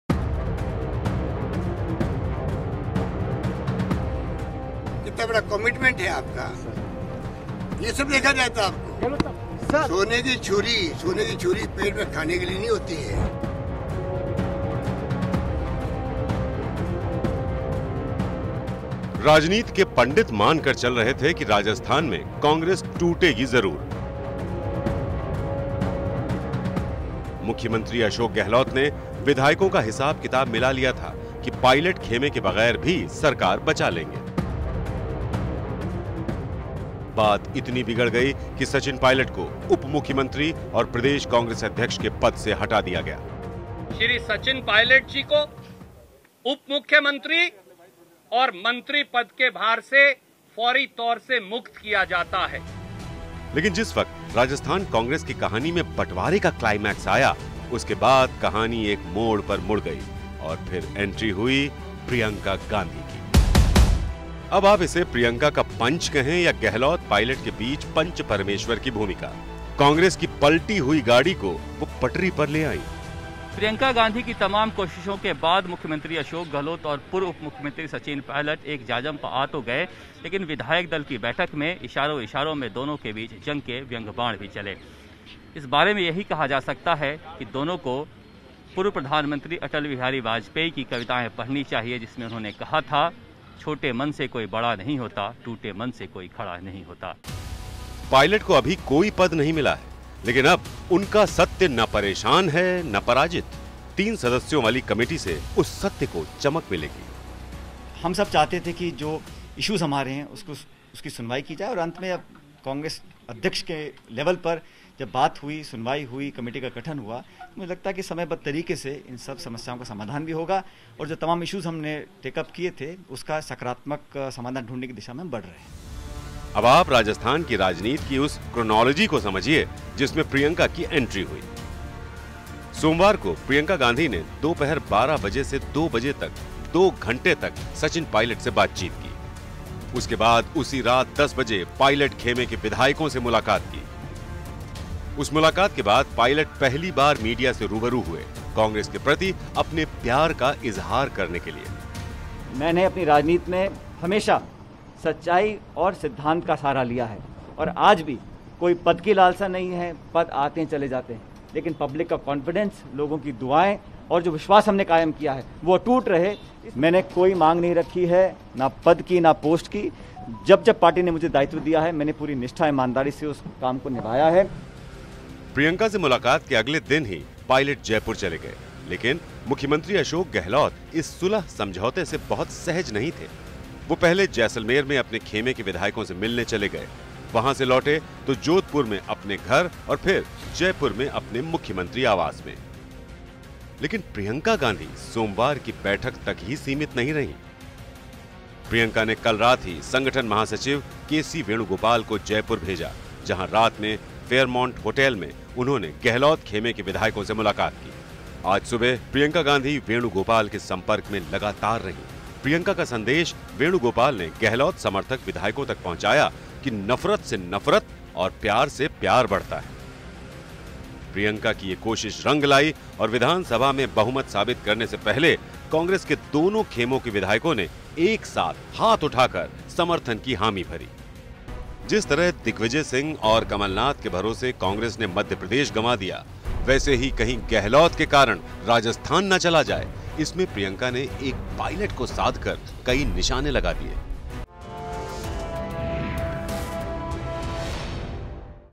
राजस्थान में PRIYANKA के 'जादू' से बच गई GEHLOT की सरकार !
सियासी मानकर ये मानकर चल रहे थे कि राजस्थान में कांग्रेस टूटेगी जरूर। सीएम अशोक गहलोत ने विधायकों का हिसाब किताब मिला लिया था कि पायलट खेमे के बगैर भी सरकार बचा लेंगे। बात इतनी बिगड़ गई कि सचिन पायलट को उप मुख्यमंत्री और प्रदेश कांग्रेस अध्यक्ष के पद से हटा दिया गया। लेकिन जिस वक्त राजस्थान कांग्रेस की कहानी में बंटवारे का क्लाइमेक्स आया, उसके बाद कहानी एक मोड़ पर मुड़ गई और फिर एंट्री हुई प्रियंका गांधी का।अब आप इसे प्रियंका का पंच कहें या गहलोत पायलट के बीच पंच परमेश्वर की भूमिका, कांग्रेस की पलटी हुई गाड़ी को वो पटरी पर ले आईं। पायलट को अभी कोई पद नहीं मिला है लेकिन अब उनका सत्य ना परेशान है, ना पराजित। तीन सदस्यों वाली कमेटी से उस सत्य को चमक मिलेगी। अब आप राजस्थान की राजनीति की उस क्रोनोलॉजी को समझिए जिसमें प्रियंका की एंट्री हुई।सोमवार को प्रियंका गांधी ने दो पहर बारह बजे से दो बजे तक दो घंटे तक सचिन पायलट से बातचीत की। उसके बाद उसी रात दस बजे पायलट खेमे के विधायकों से मुलाकात की। उस मुलाकात के बाद पायलट पहली बार मीडिया से रूबरू हुए कांग्रेस के प्रति अपने प्यार का इजहार करने के लिए। प्रियंका से मुलाकात के अगले दिन ही पायलट जयपुर चले गए लेकिन मुख्यमंत्री अशोक गहलोत इस सुलह समझौते से बहुत सहज नहीं थे। वो पहले जैसलमेर में अपने खेमे के विधायकों से मिलने चले गए। वहां से लौटे तो जोधपुर में अपने घर और फिर जयपुर में अपने मुख्यमंत्री आवास में...लेकिन प्रियंका गांधी सोमवार की बैठक तक ही सीमित नहीं रहीं, उन्होंने ऐसी रणनीति बनाई जिसके तहत राजस्थान का
सियासी झगड़ा अब लगभग पूरी तरह से सुलझ चुका है।